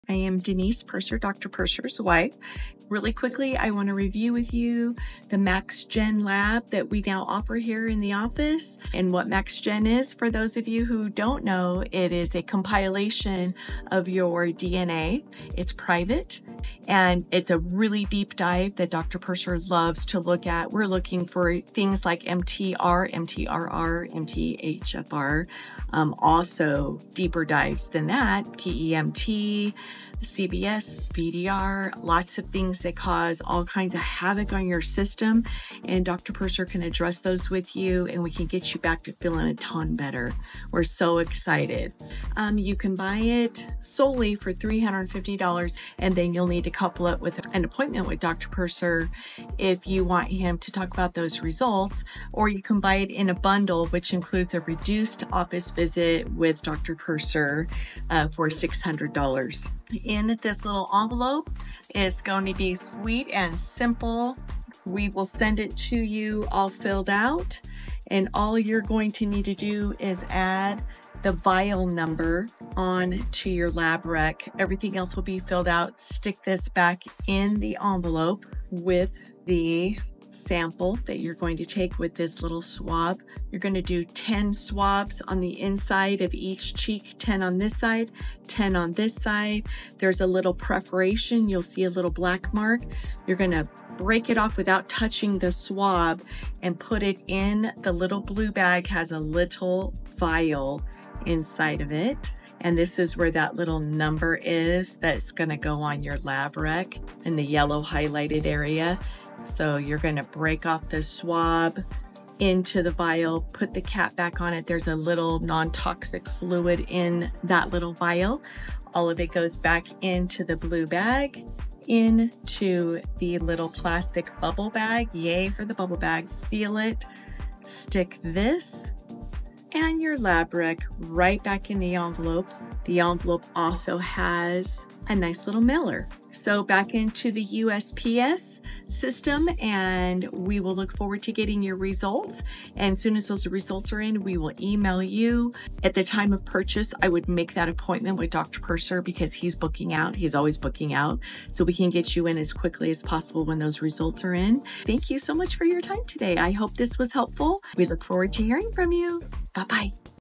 The Works - best genetic testing!
We get asked time and time again, what is the best, private, most comprehensive, and affordable genetic testing.

We finally have The Works powered by MaxGen. This testing shows all we need to get going into your personalized care.

Call my office or check my site for more info.